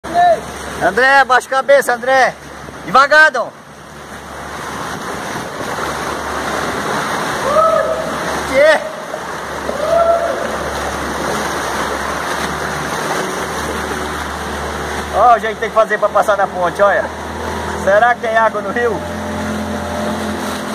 Rio Dourados volta a subir e pescador grava adrenalina de passar embaixo da ponte
VÍDEO: Rio Dourados volta a subir e pescador grava adrenalina de passar embaixo da ponte em FÁTIMA DO SUL